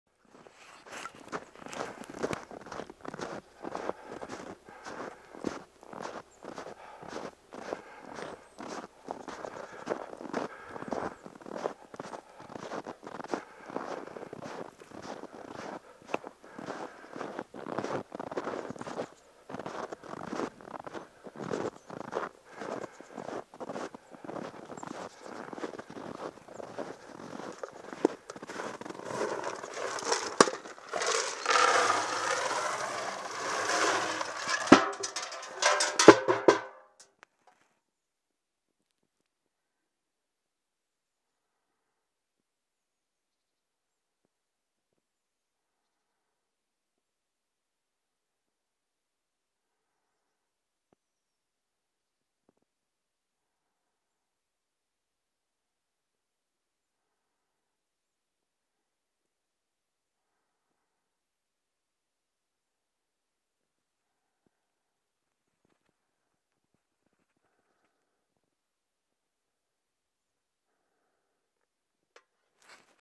Roedeers on Uljetresk
Feeding the wild Roedeers during winter. They suffer hard during our long winter with tons of snow.